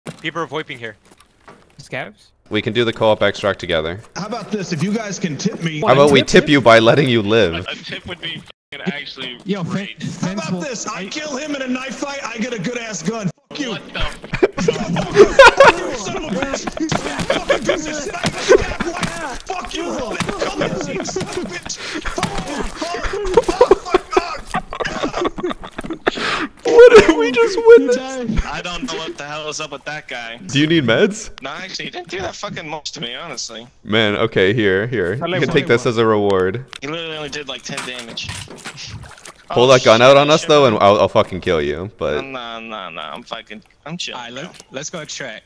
Fighting over a tip
One guy didn't want none of it